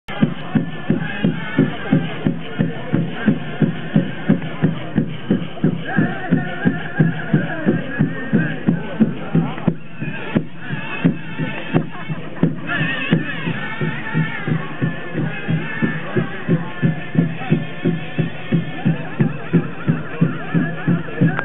Tribal Dance cont.
This young fancy dancer thrills the crowds with her coordinated moves.